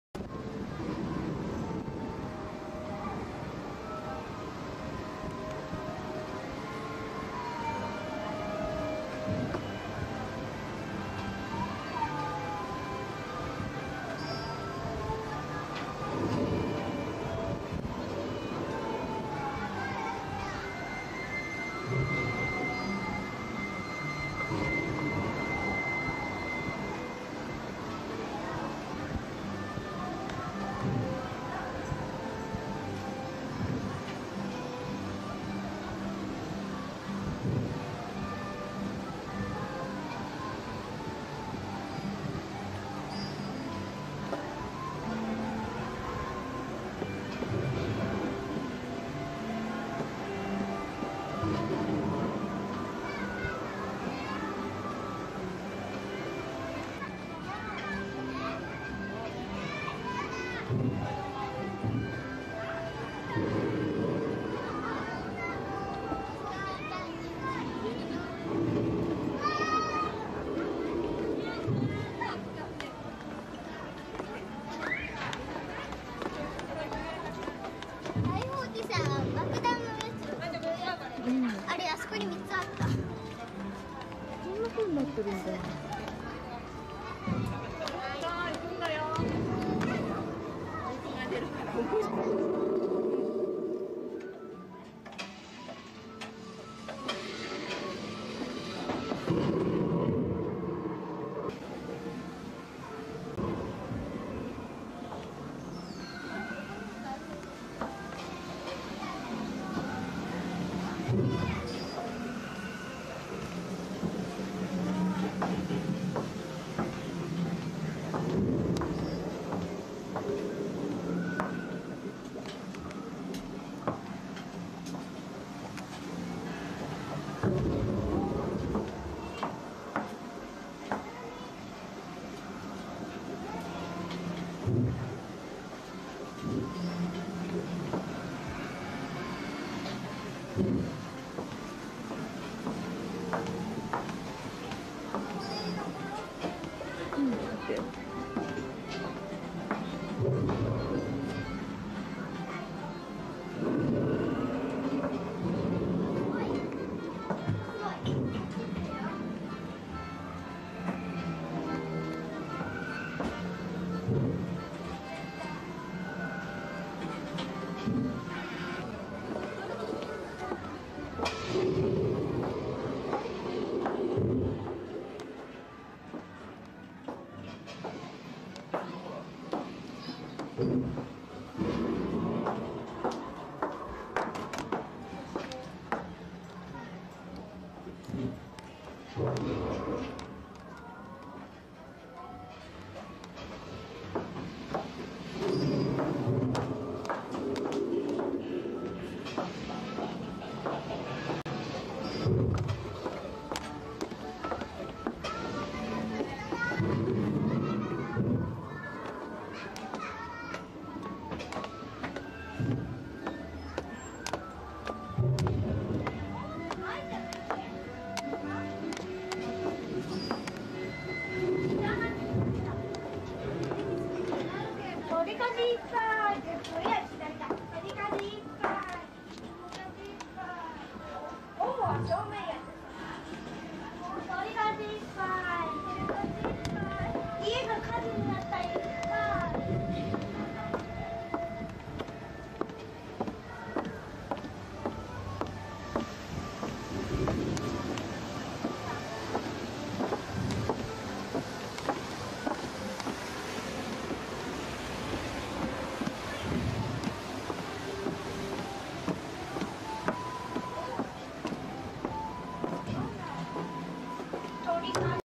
TDS 2003年6月 フォートレス・エクスプロレーションのガリオン船
東京ディズニーシー　メディテレーニアンハーバーのアトラクション
フォートレス・エクスプロレーションの中のガリオン船を少しだけ歩いてみました。